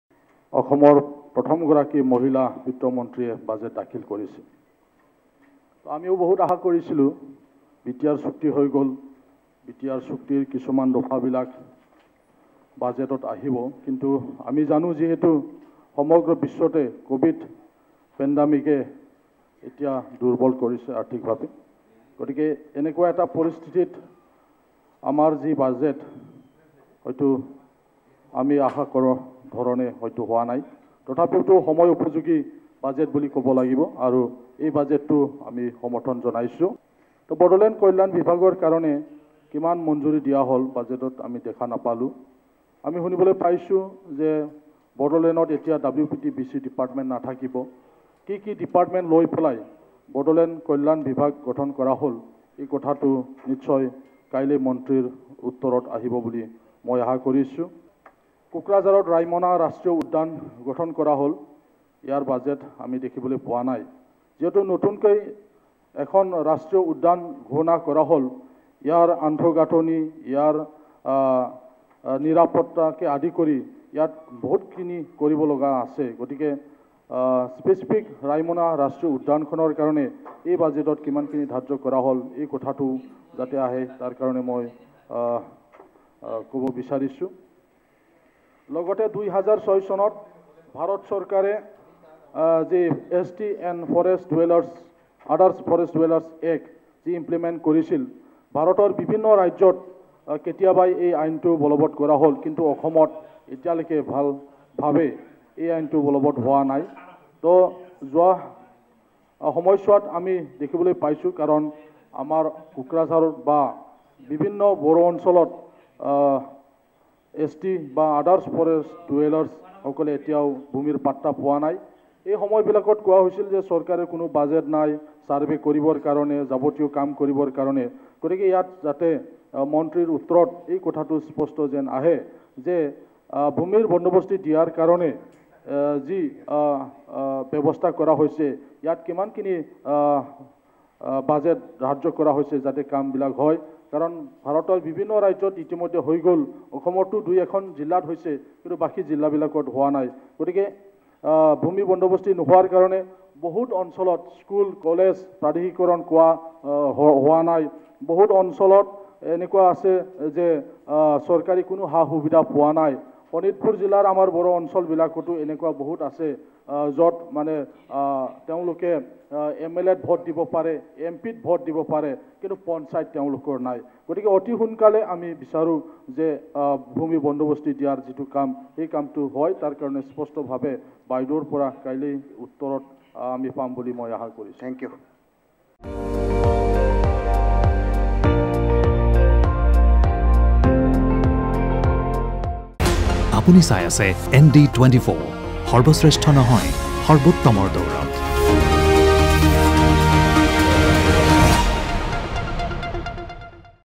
অজন্তা নেওগৰ বাজেটক লৈ অসন্তুষ্ট UPPL
BTRক গুৰুত্ব নিদিয়াৰ অভিযোগ বিজেপি মিত্ৰদল UPPLৰ
বিধানসভাৰ মজিয়াতেই বাজেটক লৈ অসন্তুষ্টি UPPL বিধায়কৰ
সমৰ্থন জনাইয়ো বাজেটক লৈ আশাহত বুলি বক্তব্য UPPL বিধায়কৰ
বিধায়ক লৰেন্স ইছলাৰীয়ে মুকলিকৈ ব্যক্ত কৰিলে অসন্তুষ্টি
আশা কৰা ধৰণে বাজেটত প্ৰতিফলন নহ'ল বিটিআৰৰ প্ৰসংগ
বড়োলেণ্ড কল্যাণ বিভাগৰ নামত ধনৰ পৰিমাণ উল্লেখ নথকাক লৈ ক্ষোভ
ৰাইমনা ৰাষ্ট্ৰীয় উদ্যানৰ নামত আৱণ্টিত ধনৰ পৰিমাণো উল্লেখ নাই বাজেটত
বন অধিকাৰ আইনৰ অধীনত ভূমি বন্দবস্তি ব্যৱস্থা কৰাৰো আহ্বান ইছলাৰীৰ